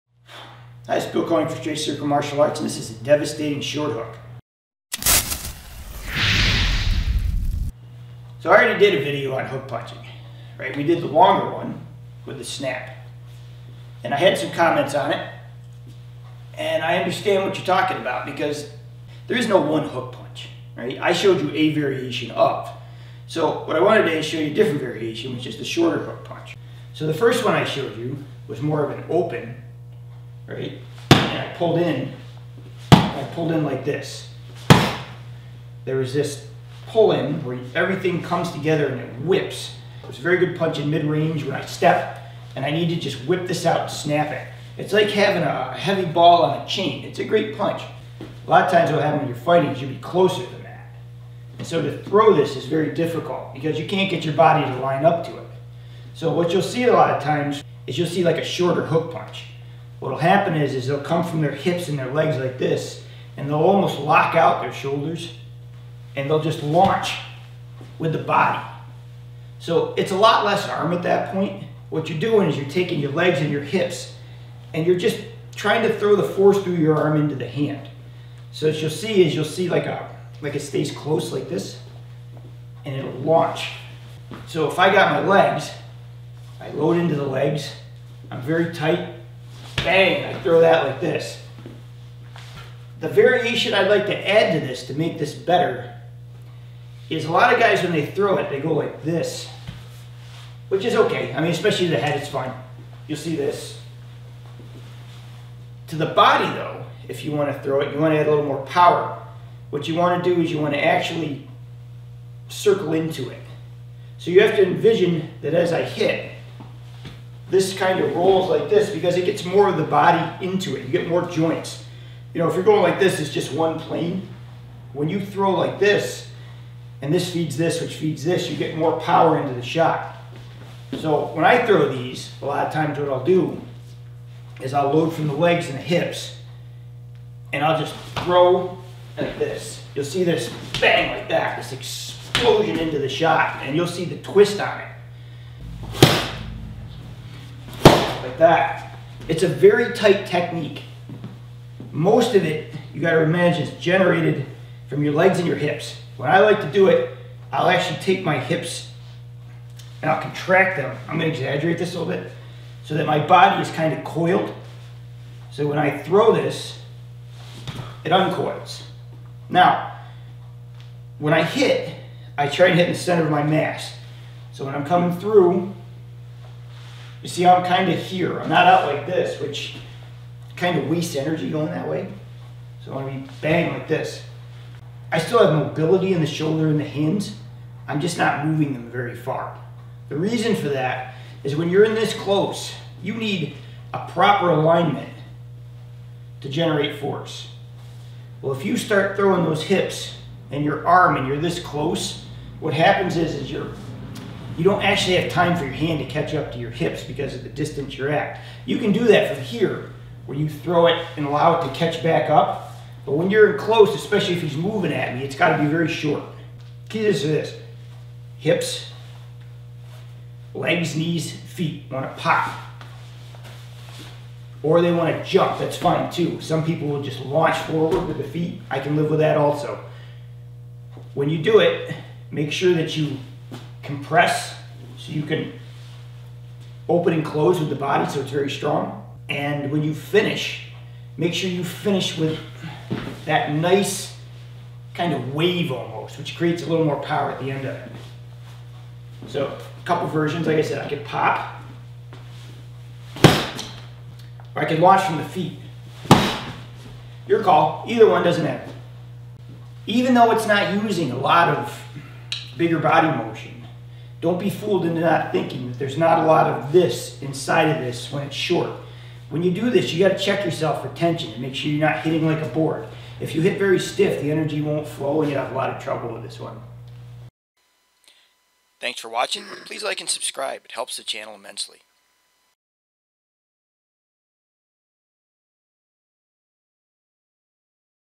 How to throw a powerful short hook.
Most hooks are thrown long. Learn how to throw it inside with power.